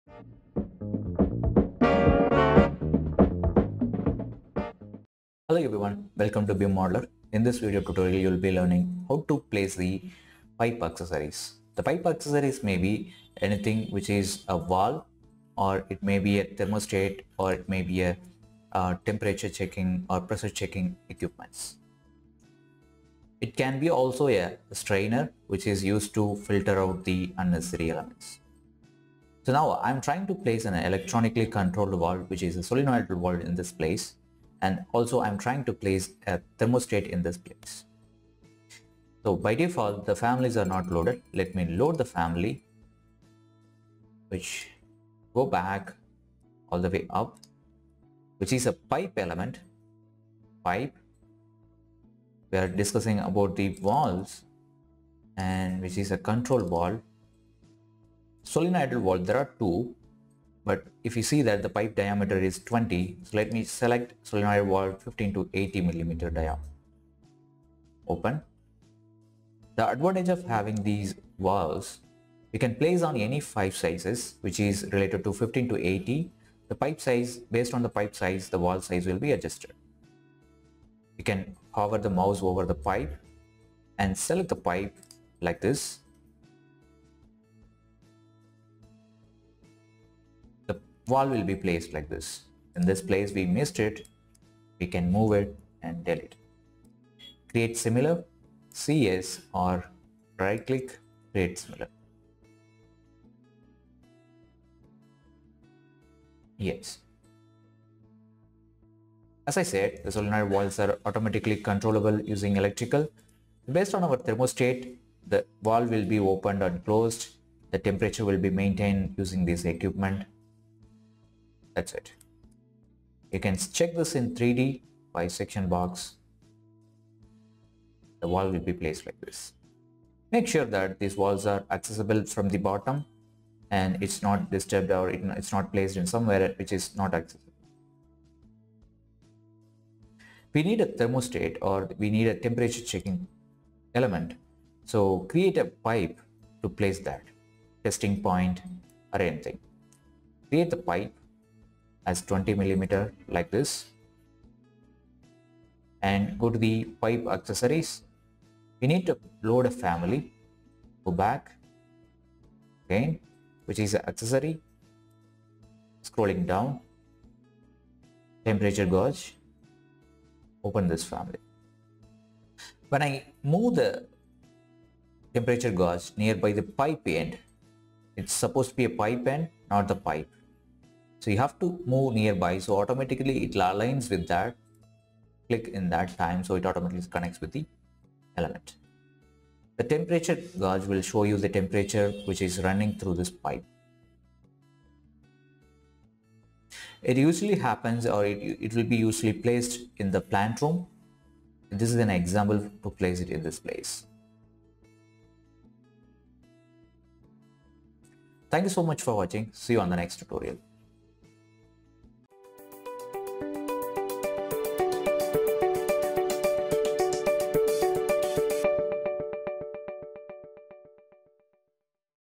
Revit MEP Project 28: How to add accessories in Revit? Valve, Filter
After placing the pipes in Revit if you are trying to place pipe valves and testing points for Temperature, pressure. The components will be under pipe accessories options. Load the families from library and use them.

Happy Learning!

For any Training & Doubt clarification session, Freelancing, Job Openings on Revit Architecture, MEP,  Dynamo, Naviswork, AutoCAD, BIM Can

Thanks!